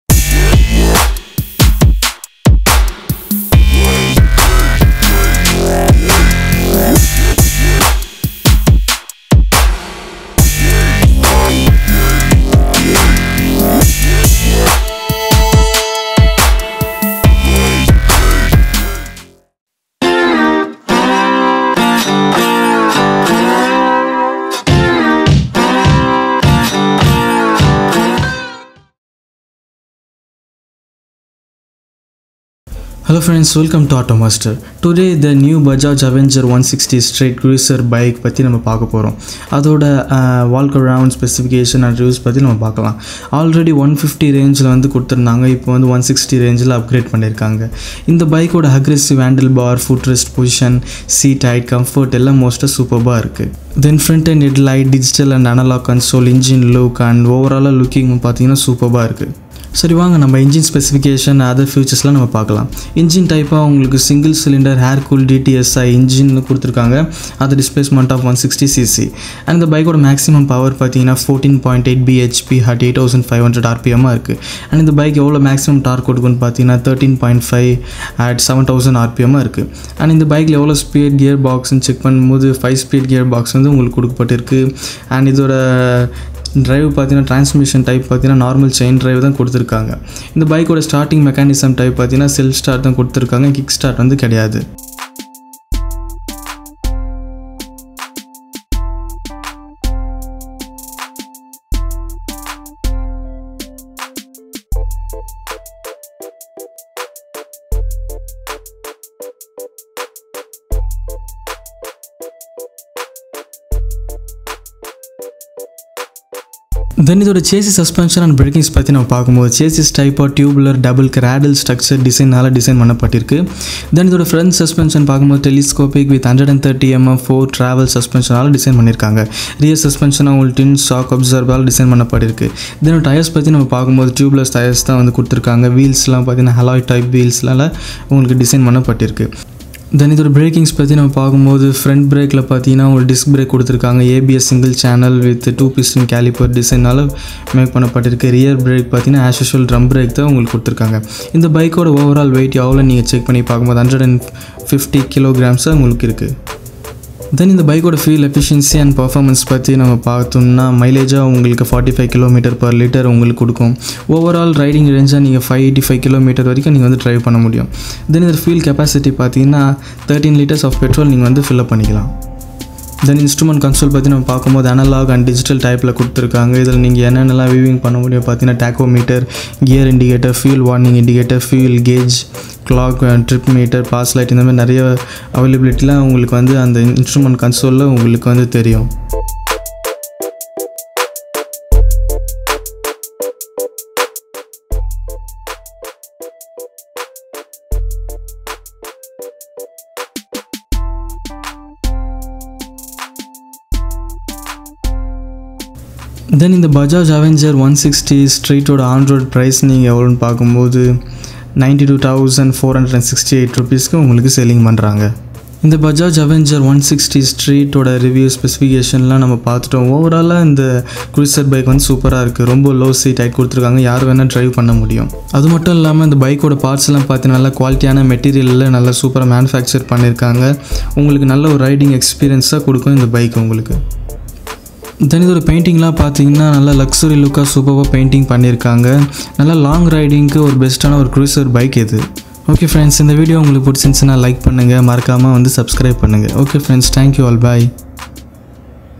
Bajaj Avenger 160 Street 2019 ABS : Full Review Tamil | Mileage
Bajaj Avenger 160 Street 2019 ABS

Helo Friends Welcome to Automaster, in This Video i have Shown the new Model bajaj avenger 160 street review price specification and walkarond view,this the one of the best bikes in cruiser model with cheap price,watch the video fully and know about this bikes,and subscribe our channel